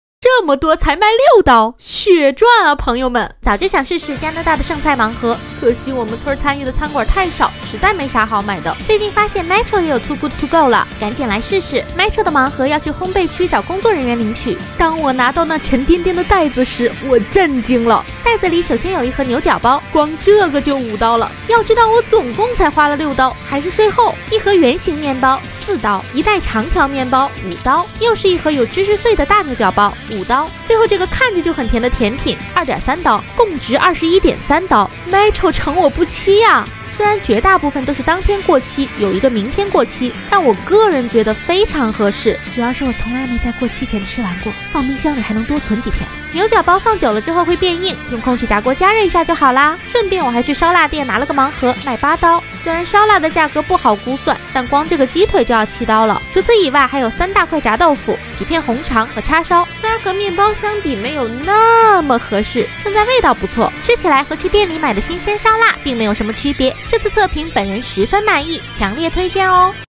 加拿大六刀的剩菜盲盒里有什么？血赚啊朋友们
多伦多薅羊毛指南
最喜欢看别人买生菜盲盒了，可惜我家附近参与的店太少了，一直没机会
这次metro上了too good to go，我终于试了一下 好合适